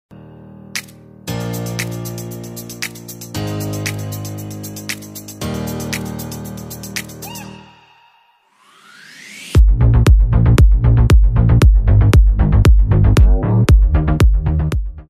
Préambule Rénovation la familia.
Première vidéo de notre chaine : Rénovation d'une maison Charentaise en habitation principale 😋.
Un petit préambule 😉.
Juste pour vous présenter rapidement le projet.